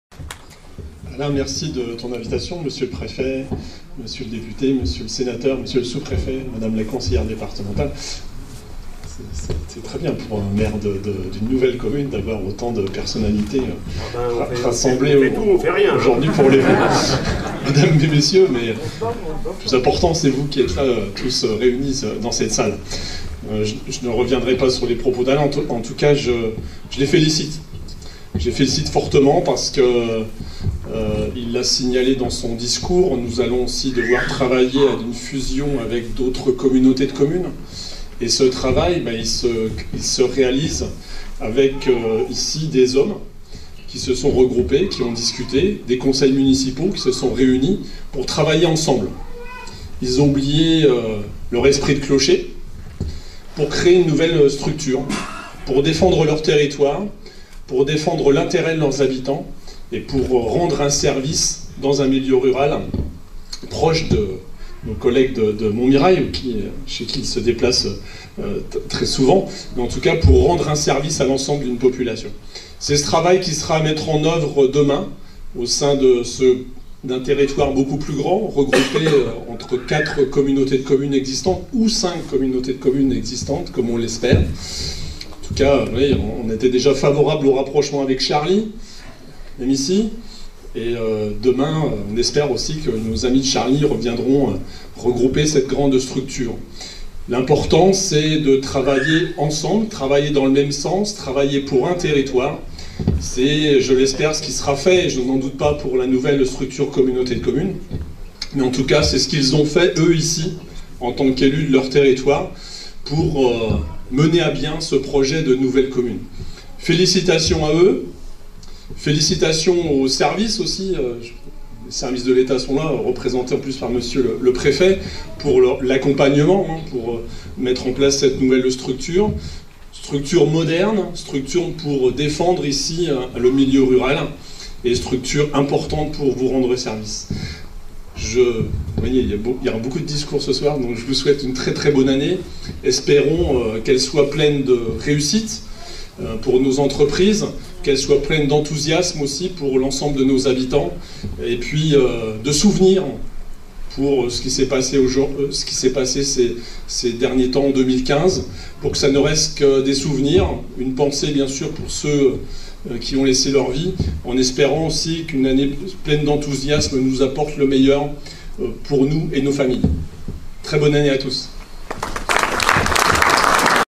Inauguration Dhuys et Morin en Brie discours Eric Mangin, président de la 4CB
Pour plus d'informations aller sur le site axomois.fr